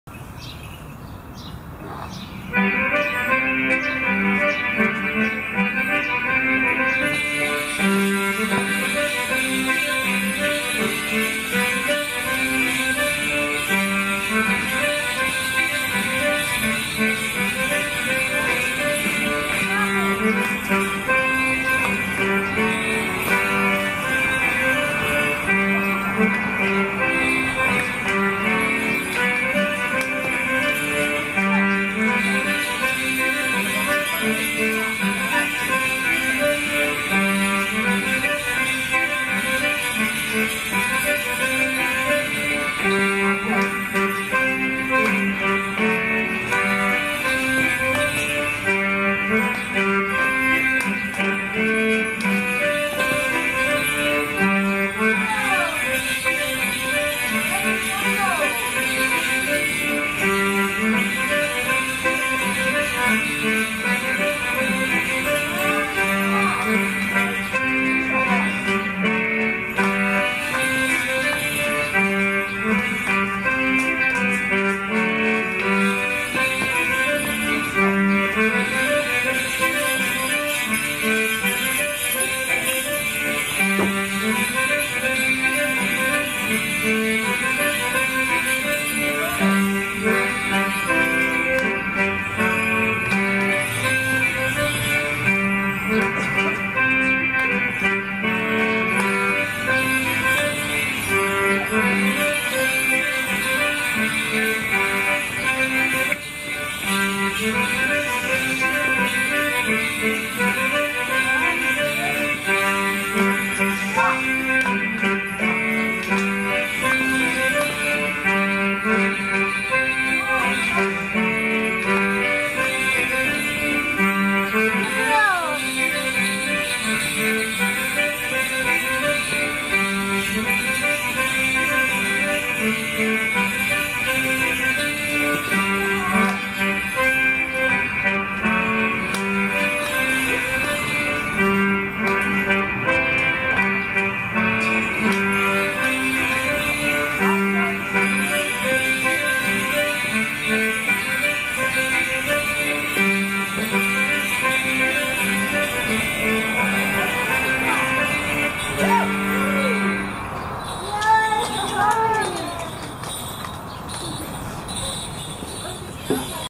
Magpie Morris Dancers teach a dance to the public Beltaine 2016
Edmonton's Magpie Morris Dancers teach a song and dance to the public in Wilbur McIntyre Park on Beltane Morning.